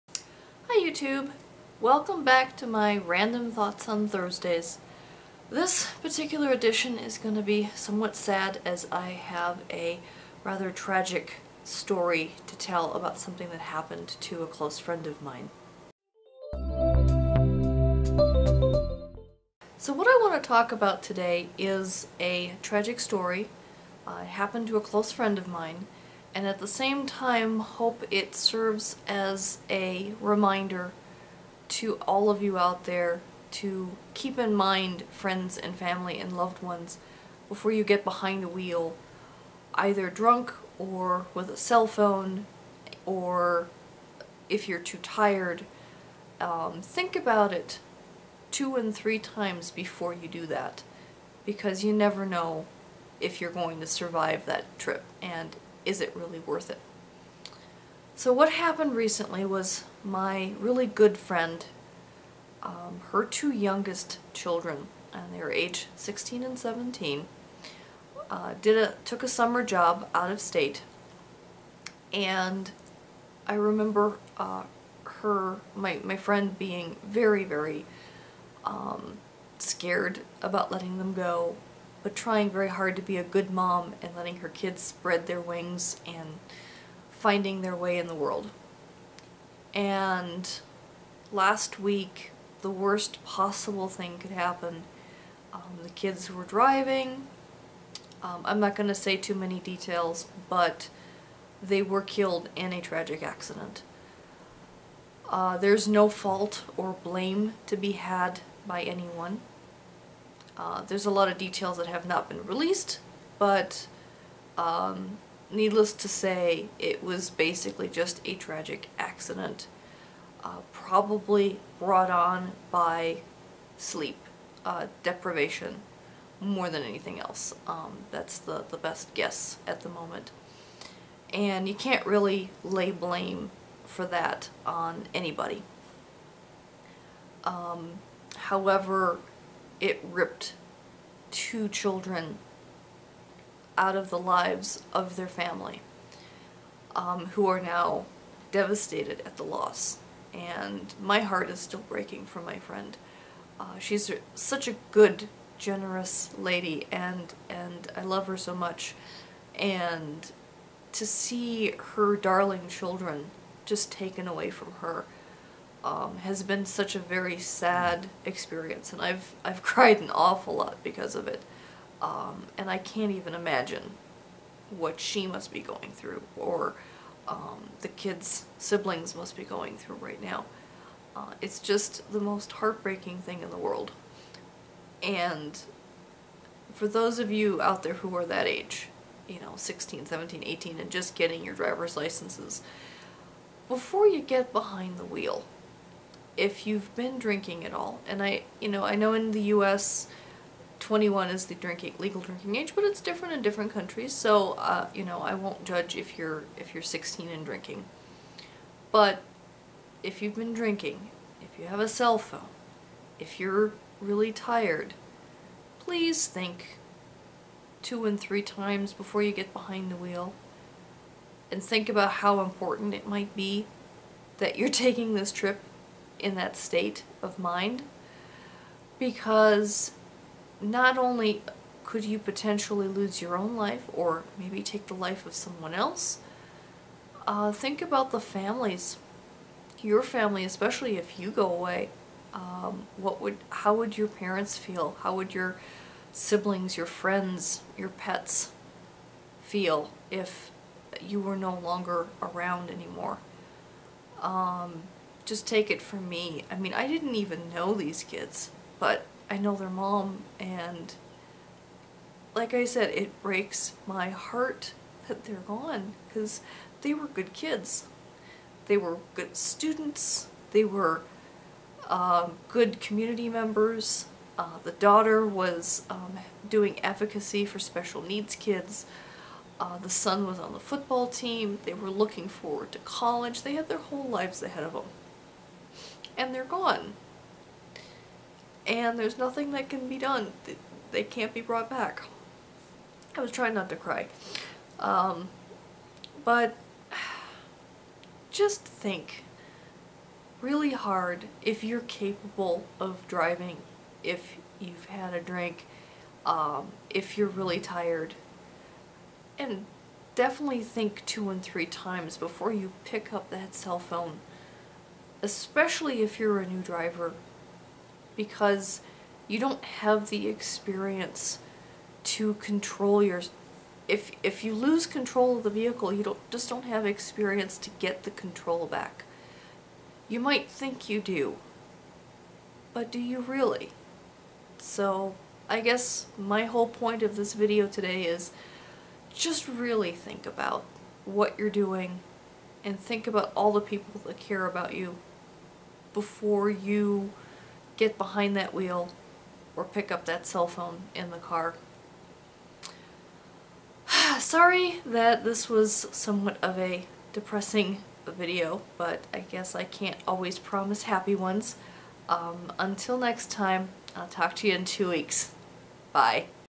Please Be Careful Out There - Random Thoughts on Thursdays
A close friend of mine lost her two youngest children in a tragic car accident. I ask everyone out there to please be careful when getting behind the wheel of a car. Don't break the hearts of the ones who love you.

Video Editing:
Adobe Premiere Pro

Video filmed on my Sony HDR-CX130